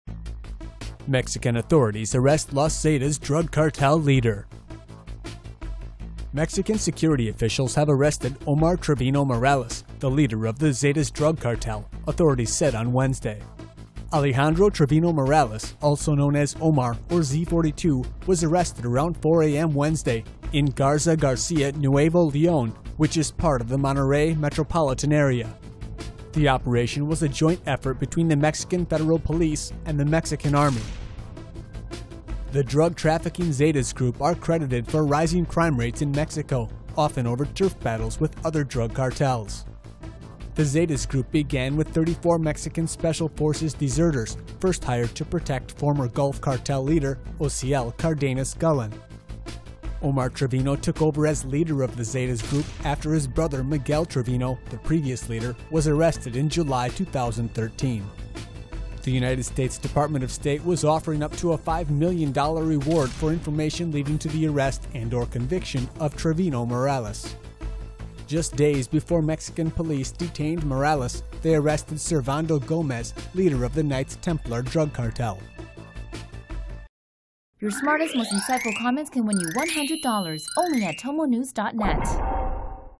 Mexican drug cartel bust: Police arrest Los Zetas leader Omar Trevino-Morales aka 'Z42'
Mexican security officials have arrested Omar Trevino-Morales, the leader of the Zetas drug cartel authorities said on Wednesday.

Alejandro Trevino-Morales, also known as "Omar" or "Z42", was arrested around 4 a.m. in Garza Garcia, Nuevo Leon—which is part of the Monterrey metropolitan area. The operation was a joint effort between the Mexican Federal Police and the Mexican Army.

The drug trafficking Zetas group are credited for rising crime rates in Mexico, often over turf battles with other drug cartels. The Zetas group began with 34 Mexican Special Forces deserters—first hired to protect former Gulf Cartel leader Osiel Cardenas-Guillen.

Omar Trevino took over as leader of the bloody Zetas group after his brother Miguel Trevino, the previous leader, was arrested in July 2013. The United States Department of State was offering up to a $5 million dollar reward for information leading to the arrest and/or conviction of Trevino Morales.

Just days before Mexican police detained Morales, they arrested Servando Gomez, leader of a the Knights Templar drug cartel.